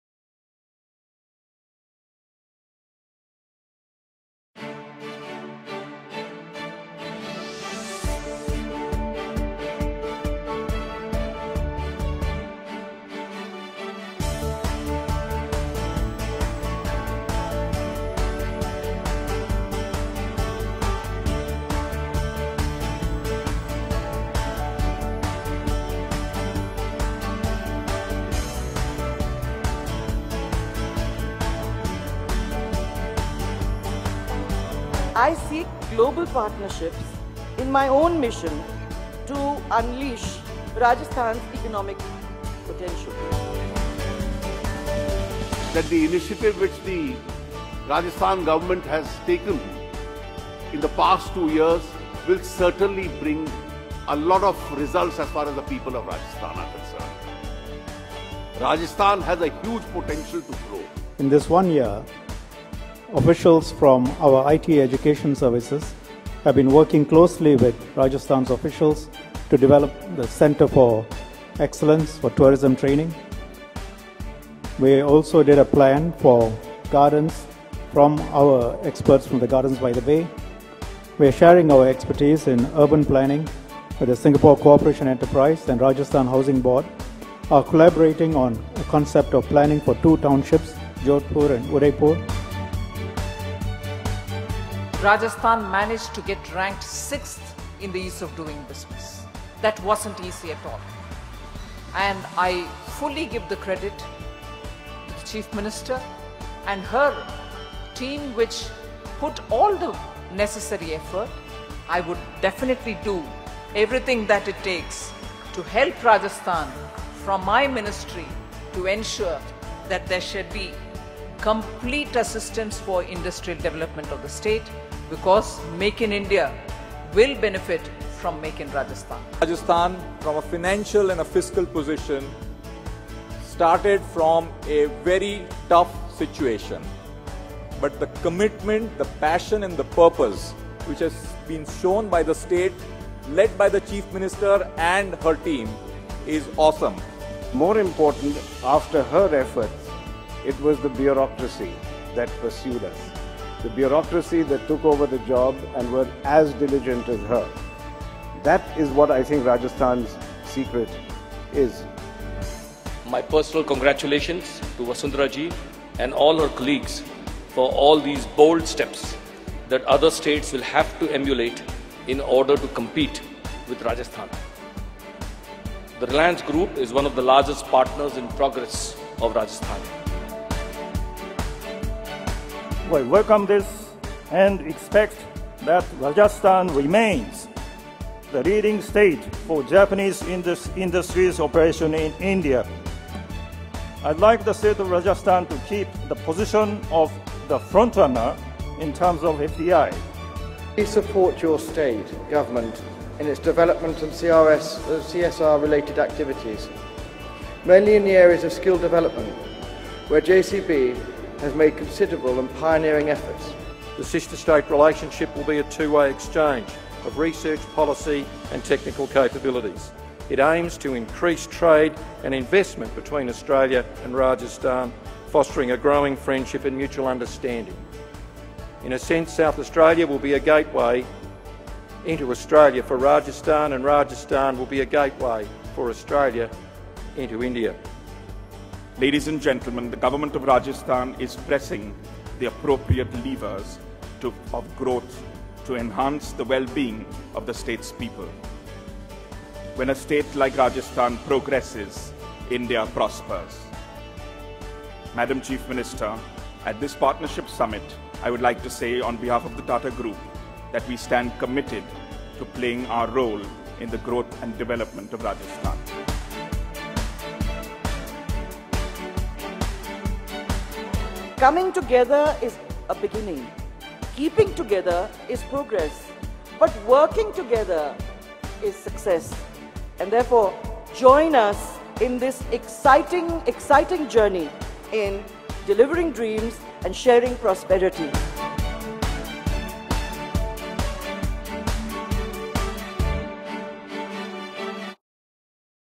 RR Glipmses Film
We are committed to providing a significantly better and more prosperous life to all the citizens of the State. For people of Rajasthan to realise their dream of a much better life for themselves and their children, it is essential that we create an entire ecosystem of opportunities

The MoUs signed with companies across sectors during ‘Resurgent Rajasthan’.